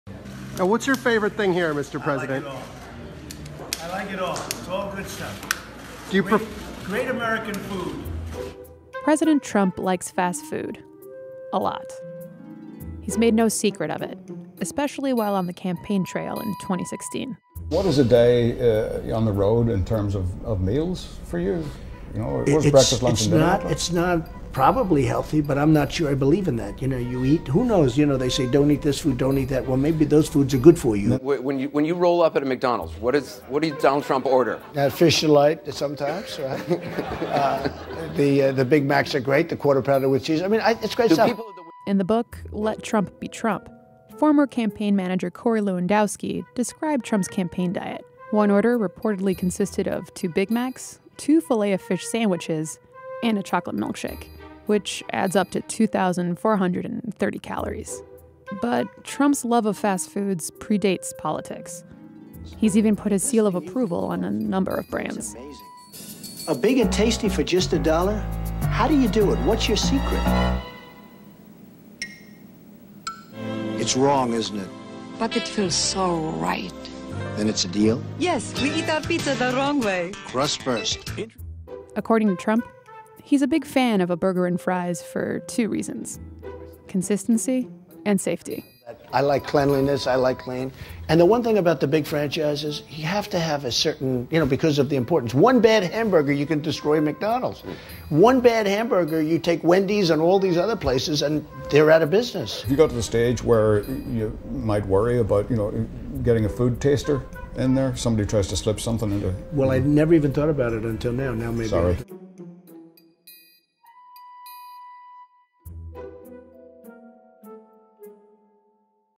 Trump’s love of fast food, explained
President Trump’s love of fast food predates politics. His reasons for loving big name brands like McDonald’s, Wendy’s and Burger King are a bit complicated.